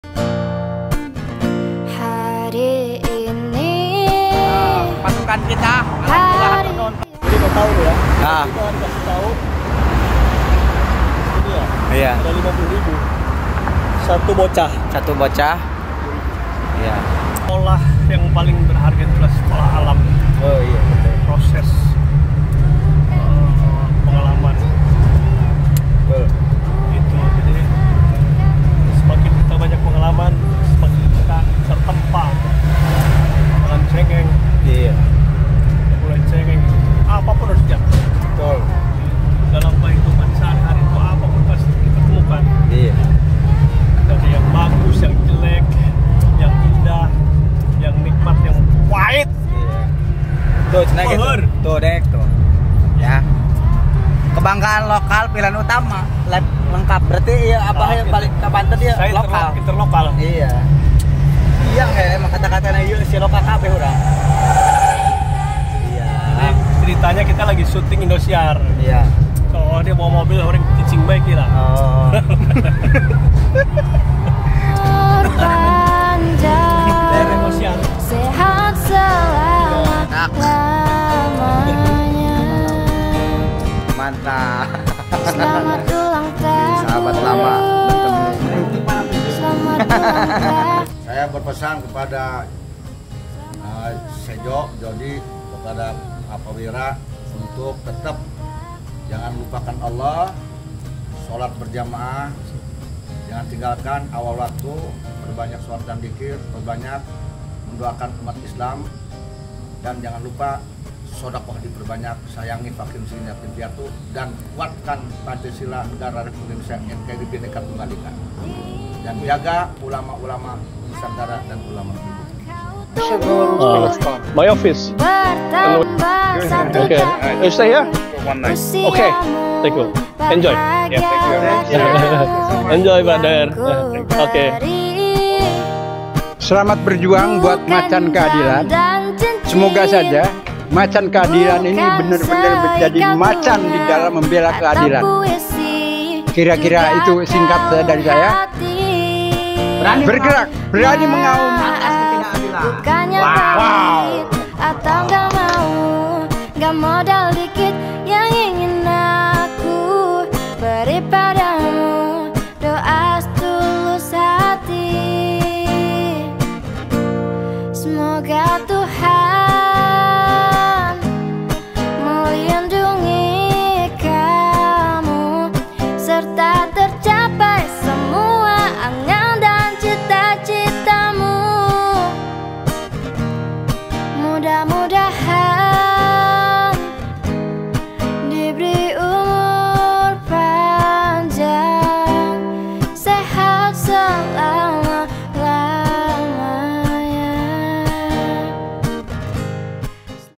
Selamat Ulang Tahun Abah Lawyer Nandang Wirakusumah Pembina Macan Keadilan Indonesia
Selamat ulang tahun Abah Lawyer Nandang Wirakusumah Nandang Wirakusumah (Dewan Pembina Macan Keadilan Indonesia) sehat selalu, tetap semangat memperjuangkan keadilan untuk wong cilik!